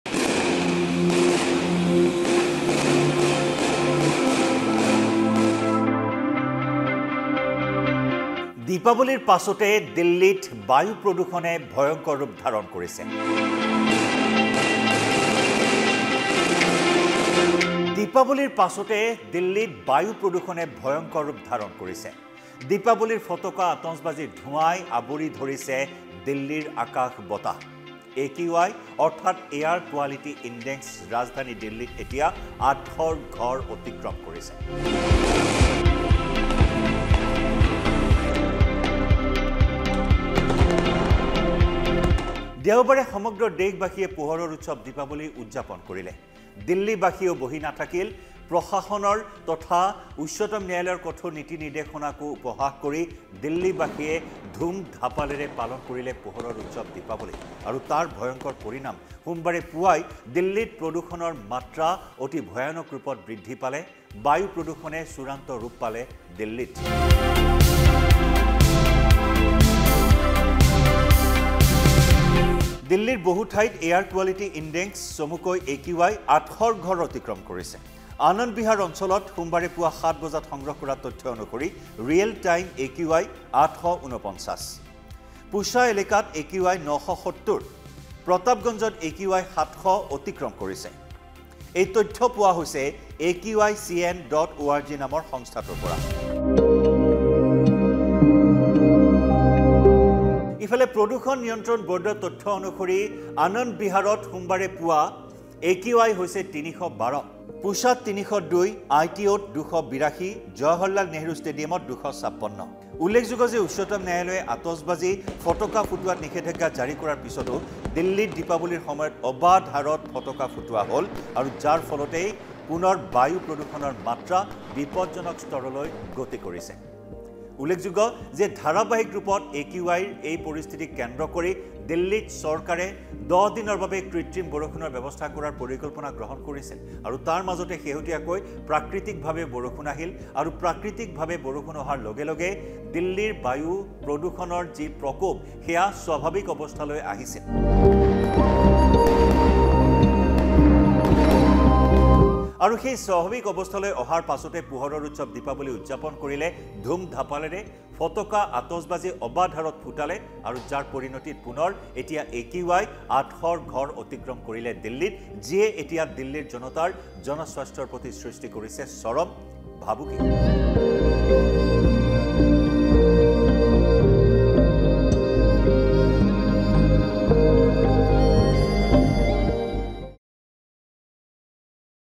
Delhi Pollution in Diwali Festival 2023 : পোহৰৰ উৎসৱে দেশৰ ৰাজধানীলৈ নমাই আনিছে অমানিশা | N18V
Delhi Pollution in Diwali Festival 2023 | পোহৰৰ উৎসৱে দেশৰ ৰাজধানীলৈ নমাই আনিছে অমানিশা
দীপাৱলীৰ পিছতে পুনৰ ভয়ংকৰ প্ৰদূষণে গ্ৰাস কৰিছে দিল্লী
আতচবাজীৰ ধোঁৱাই আৱৰি ধৰিছে দিল্লীৰ আকাশ বতাহ
দিল্লীৰ বহু ঠাইত AQI ৮০০ৰ ঘৰ অতিক্ৰম
নিষেধাজ্ঞাৰ মাজতো দিল্লীত অবাধ হাৰত ফুটালে ফটকা